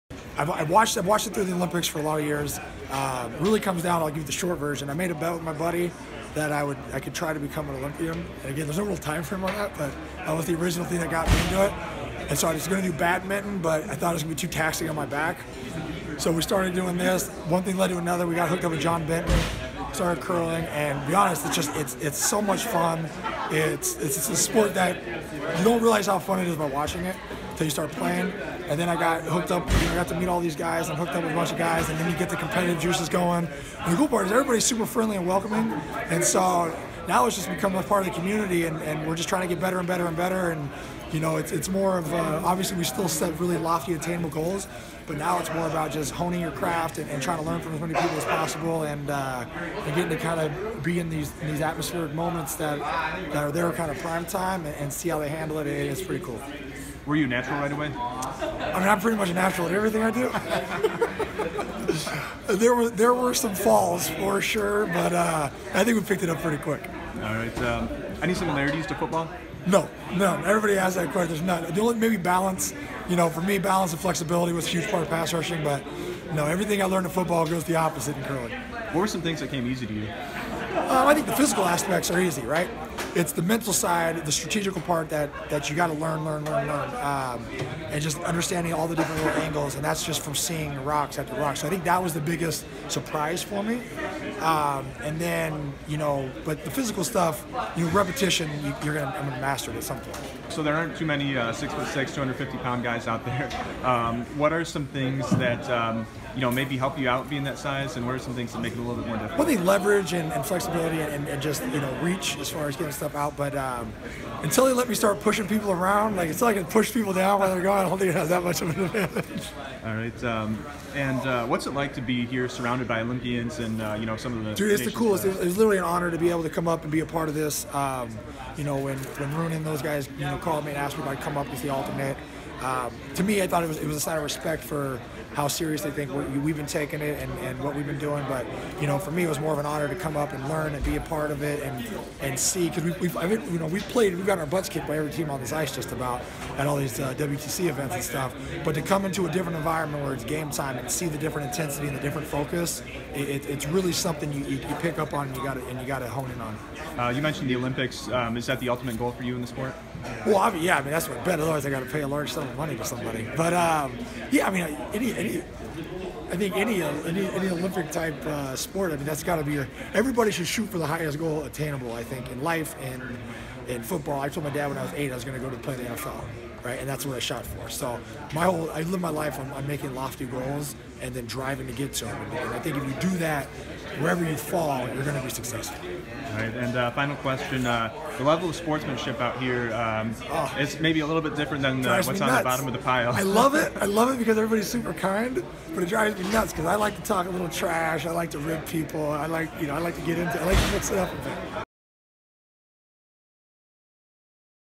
Jared Allen talks Olympic curling goal and why he misses NFL trash talk
Jared Allen discusses his transition from NFL Pro Bowler to professional curling during the USA Curling Nationals in Kalamazoo, Michigan on Monday, Feb. 11, 2019.